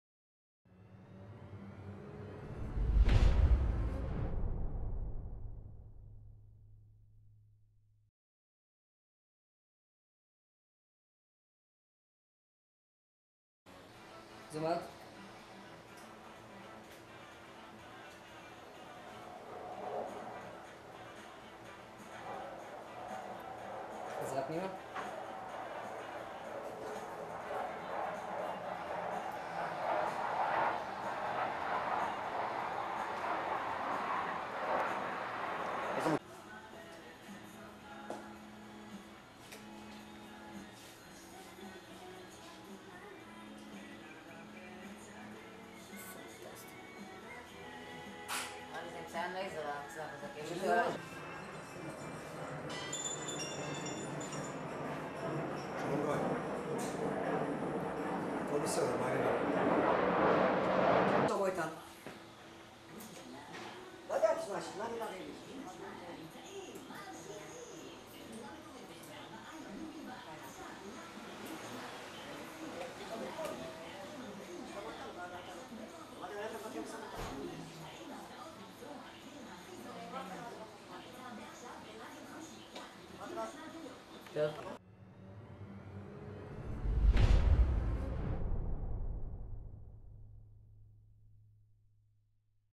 Shanny X20 Calibration - short.mpg
Shanny is a  continous optical X20 sight for remote controlled weapon stations and guns. Due to its superb optical design, the Shanny can detect a passing car at a range of over 18 Km.